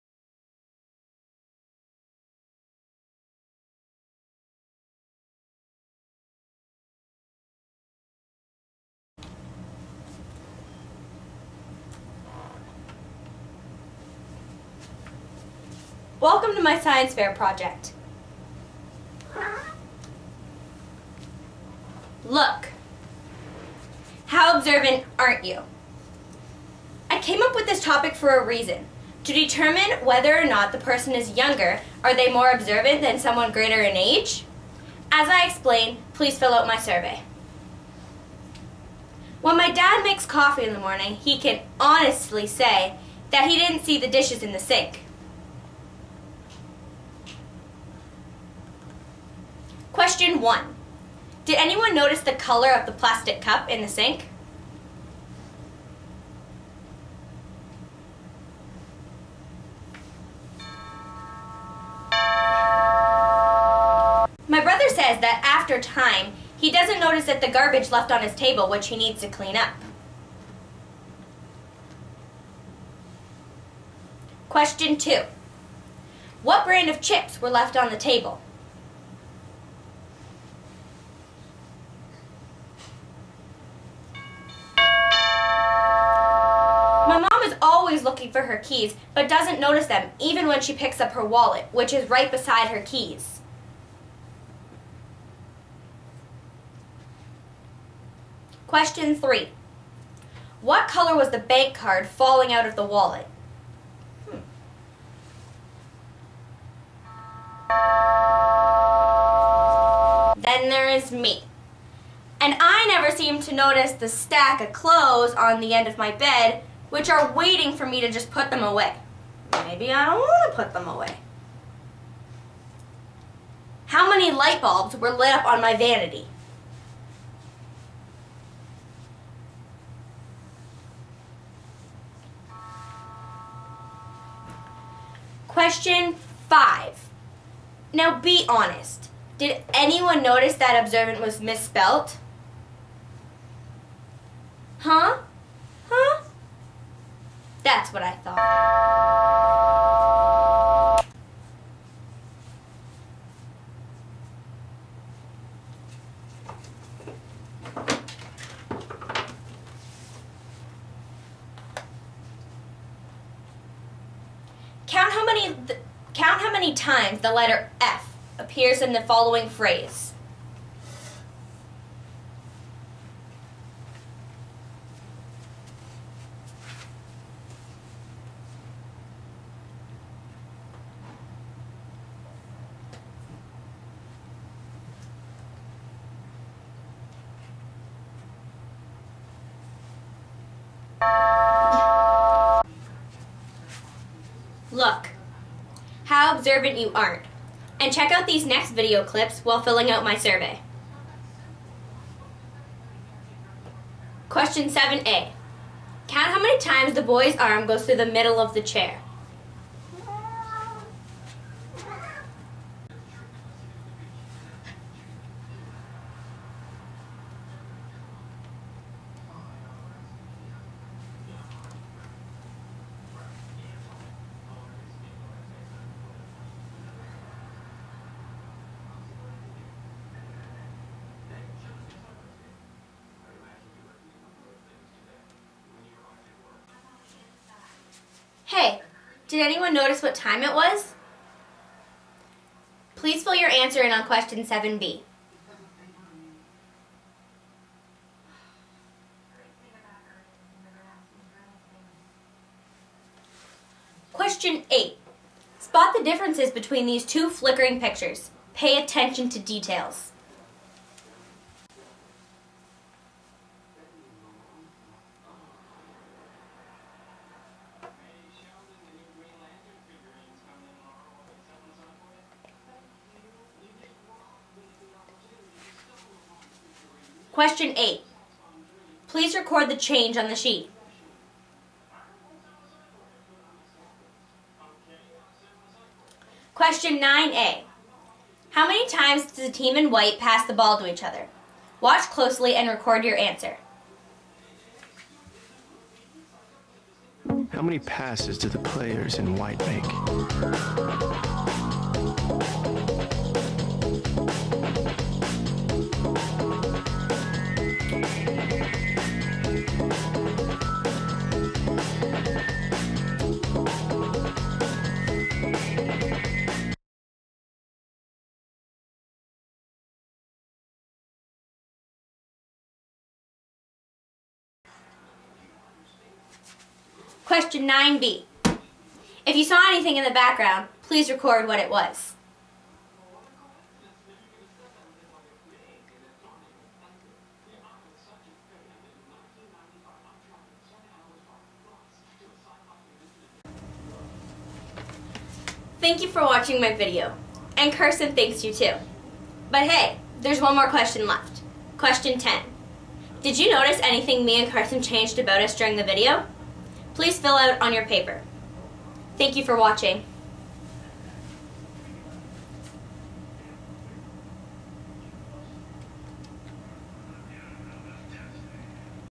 LOOK, How Observant Aren't You?
Grade 7 Science Fair video experiment.  Student needs various age categories to test participants to see who is the most or least observant according to their age.  You must view the still images and video clips along with the narration of the student and answer the questions.  The student also has another set of questions of "obvious everyday" visuals that accompanies her experiement.  She is targeting 100 people in 5 age categories, 10 - 19, 20 - 29, 30 - 39, 40 - 49 and 50+ to determine if the younger or older are most observant.